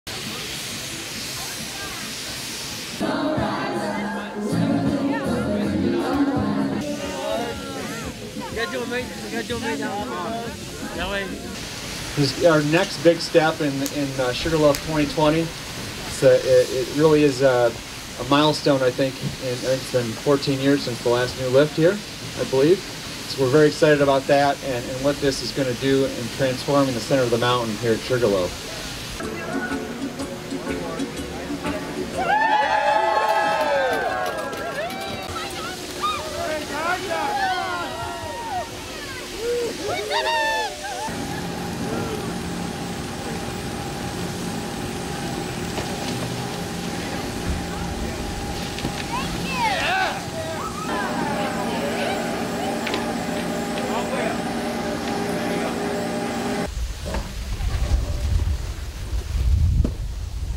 Sugarloaf Skyline Quad Grand Opening B-Roll
Footage of today's Grand Opening ceremonies for Sugarloaf's new Skyline Quad.